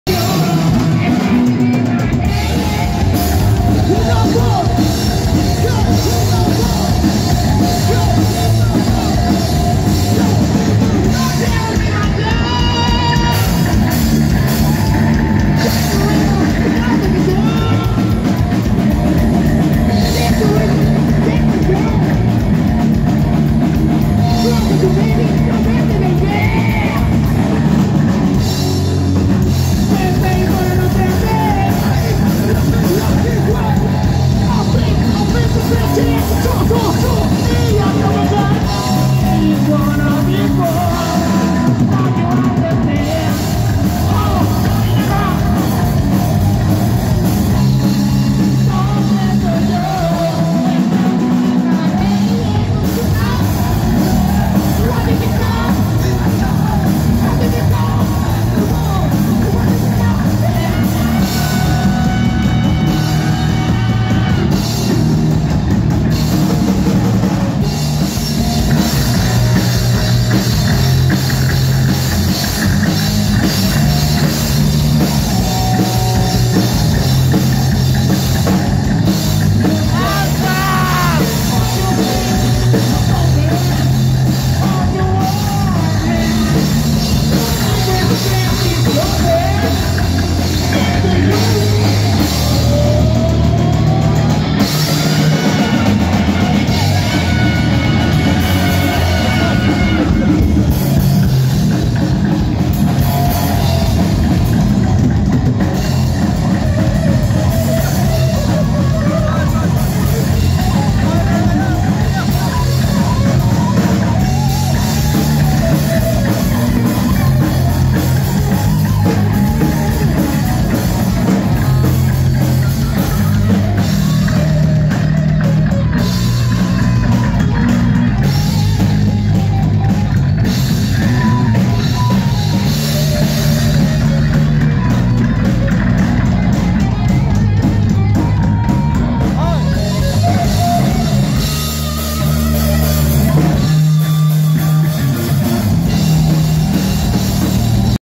HELL PATROL LIVE Chiclayo 2024
HELL PATROL EN VIVO LOCAL LA TABERNA COVER DE ANTHRAX